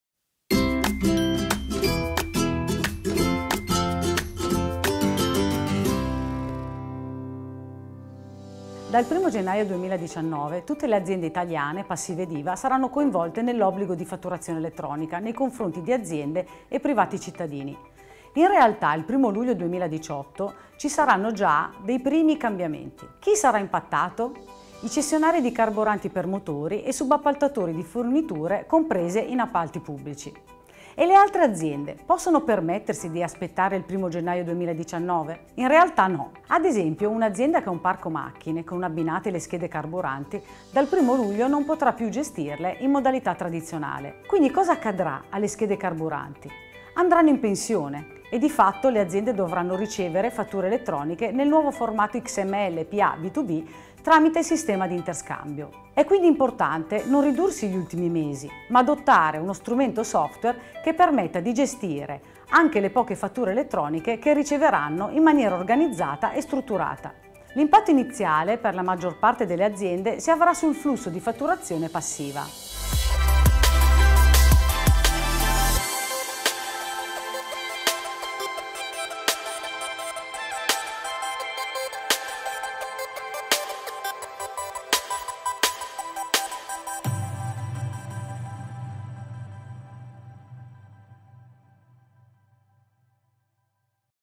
Impatti della Fatturazione Elettronica sulle aziende
Chi viene impattato dall'entrata in vigore del 1 luglio 2018? Quando le aziende devono attivarsi?